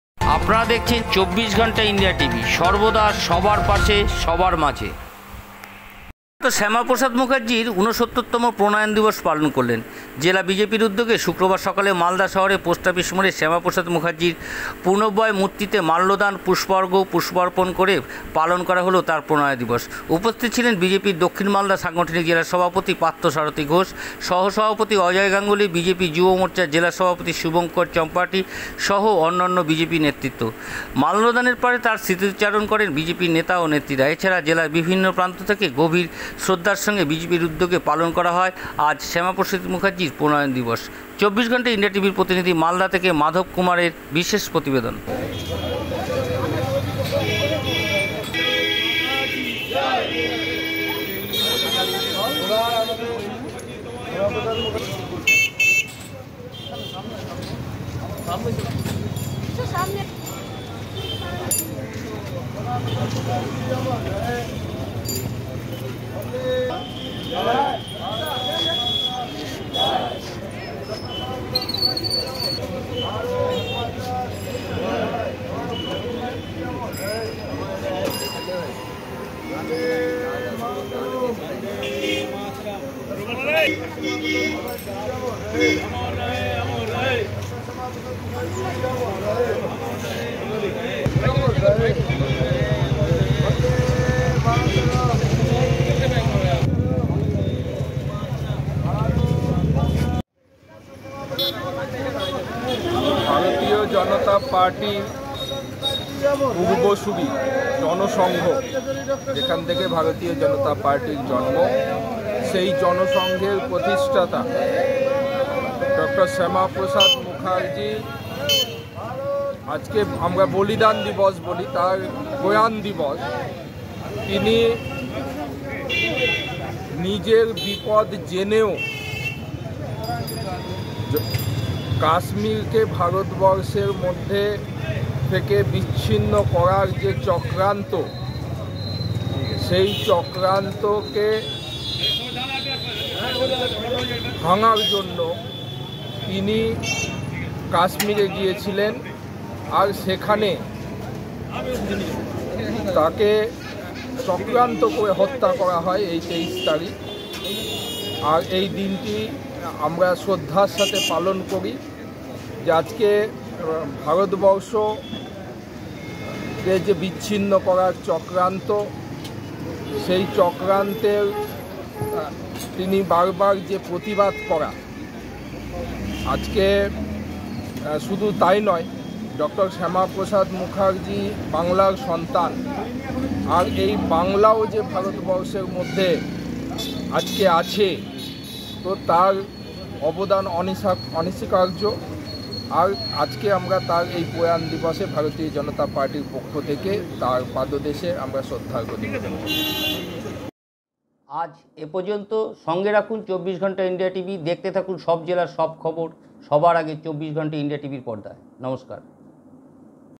প্রয়াত শ্যামাপ্রসাদ মুখার্জির ৬৯ তম প্রয়াণ দিবস পালন
-প্রয়াত শ্যামাপ্রসাদ মুখার্জির ৬৯ তম প্রয়াণ দিবস পালন। জেলা বিজেপির উদ্যোগে শুক্রবার সকালে মালদা শহরের পোস্ট অফিস মোড়ে শ্যামাপ্রসাদ মুখার্জির পূর্ণবয়ব মূর্তিতে মাল্যদান ও পুষ্পার্ঘ্য অর্পণ করে পালন করা হয় প্রয়াণ দিবস।
উপস্থিত ছিলেন, বিজেপির দক্ষিণ মালদা সাংগঠনিক জেলার সভাপতি পার্থসারথি ঘোষ, সহ-সভাপতি অজয় গাঙ্গুলী, বিজেপি যুব মোর্চার জেলা সভাপতি শুভঙ্কর চম্পটি সহ অন্যান্য বিজেপি নেতৃত্ব।
মাল্যদানের পর স্মৃতিচারণ করেন বিজেপির নেতা-নেত্রীরা। এছাড়াও জেলার বিভিন্ন প্রান্তে গভীর শ্রদ্ধার সঙ্গে বিজেপির উদ্যোগে পালন করা হয় শ্যামাপ্রসাদ মুখার্জির প্রয়াণ দিবস